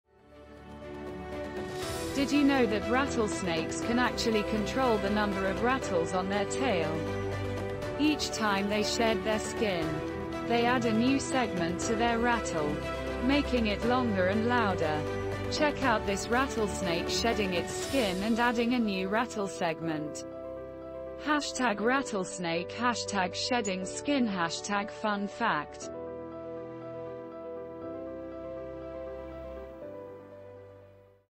Rattlesnake Fact
In this video, you'll learn a fun fact about the RATTLESNAKE. Did you know Did you know that rattlesnakes can actually control the number of rattles on their tail? Each time they shed their skin, they add a new segment to their rattle, making it longer and louder. Check out this rattlesnake shedding its skin and adding a new rattle segment! rattlesnake sheddingskin funfact?